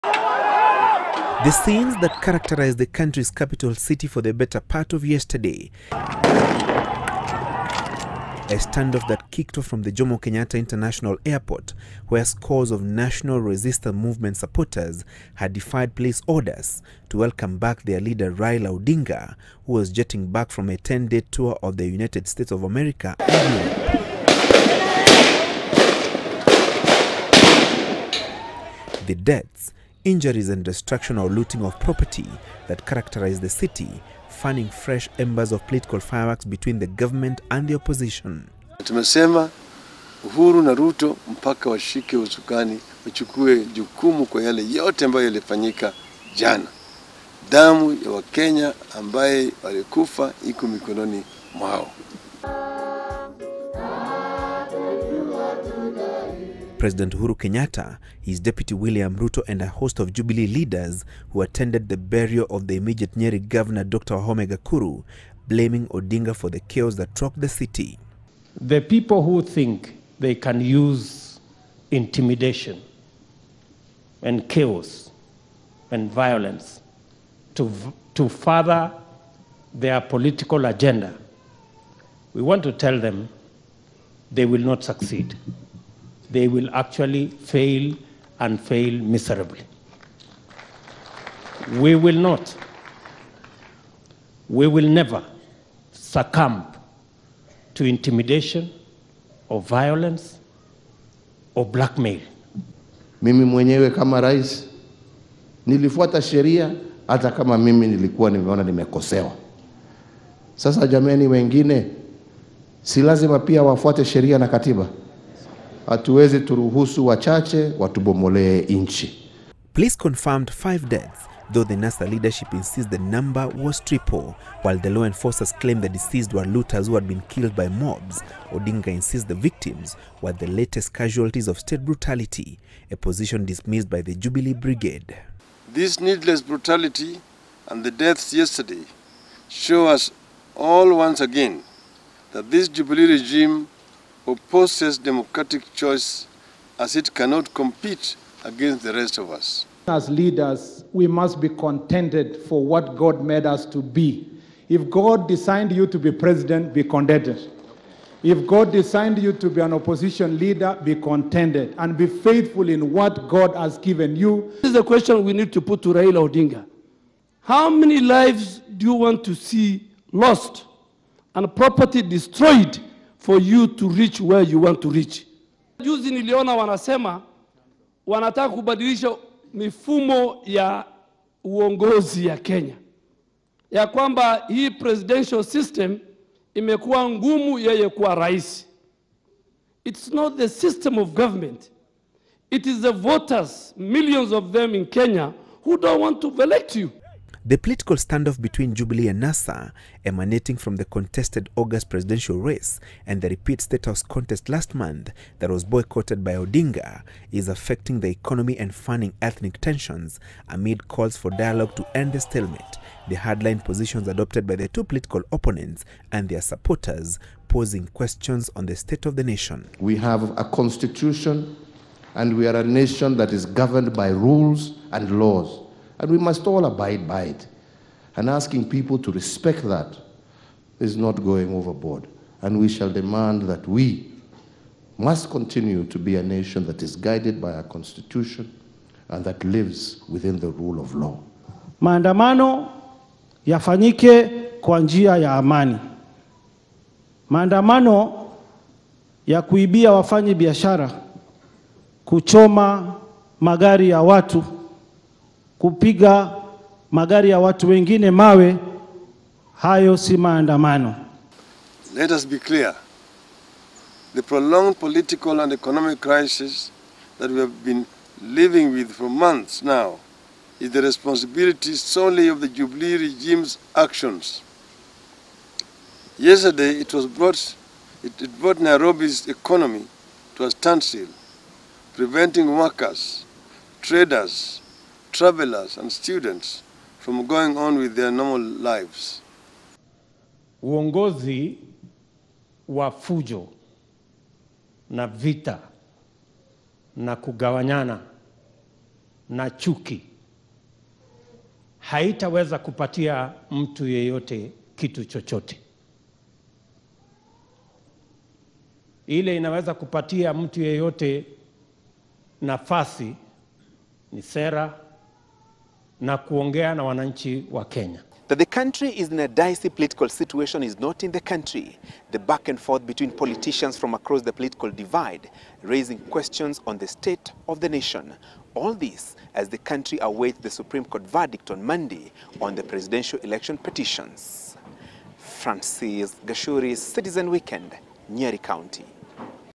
Kenyatta, Odinga trade blame over Friday chaos
The government and the opposition are locked in a blame game over yesterday’s violence in Nairobi that left at least 5 people dead, scores of injuries and massive destruction of property. While President Uhuru Kenyatta and his deputy William Ruto blame Raila Odinga for the chaos, the NASA leader accuses the government of triggering the violence by unleashing police on his supporters.